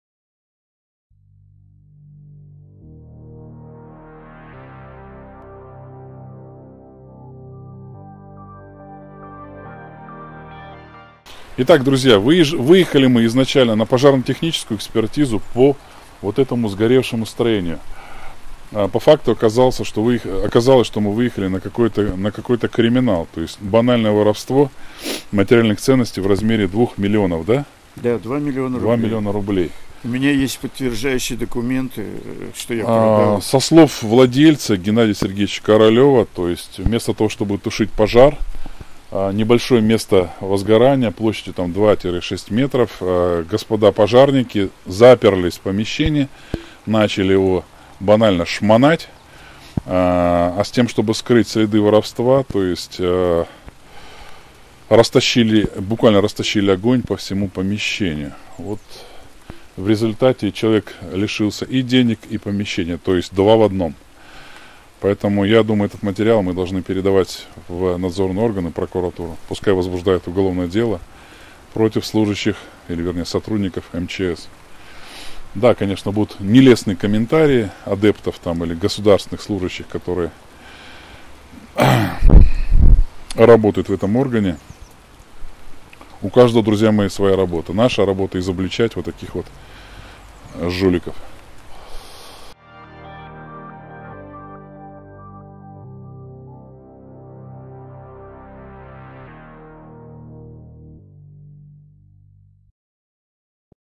🔴 Пожар в доме ветерана. Часть3. Криминал? // Пожарная экспертиза
15 апреля 2018 года произошёл пожар в жилом доме в селе Ельдигино, Пушкинского района. По сообщению сетевого издания «Маяк»:

"Произошло возгорание частного двухэтажного дома, на четырех хозяев размером 10х20 метров. «В результате пожара дом выгорел на половине площади. Удалось избежать распространения огня на соседние строения», - говорится в сообщении. Погибших и пострадавших нет. Пожар тушили 4 единицы техники и 14 человек, добавляется в сообщении."

Почему это дело вызвало наш глубочайший интерес? Здесь было несколько причин:
1)Пострадавший в пожаре - ветеран 85 лет.
2)В результате пожара было потеряно всё имущество и место жительства.
3)В результате судебных разбирательств появился судебный иск на самого ветерана.
4)В процессе диалога выясняются неожиданные подробности
5)Жалобы ветерана до пожара остались неуслышанными

Мы не могли пройти мимо такой ситуации и на безвозмездной основе решили принять участие в этом деле.

Съёмочная группа в составе эксперта в области пожарно-технических, инженерно-технологических экспертиз Федерации Судебных Экспертов - Григорьева Владимира Владимировича, директора НП "Федерации Судебных экспертов" - Костюченко Василия Владимировича и видеооператора выехала на место пожара.

В третьей части мы наблюдаем масштабы последствий произошедшего и убеждаемся что выехали отнюдь не на банальную пожарно-техническую экспертизу.

Во второй части мы осмотрели место пожара и узнали много странных обстоятельств как о причинах возгорания, как о его тушении, так и последствиях этого пожара.

В первой части по пути следования на место пожара мы обсуждали имеющуюся по данному делу информацию.

тел: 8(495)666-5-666 (Телефон для жителей Москвы и МО)

✎ Оставляйте комментарии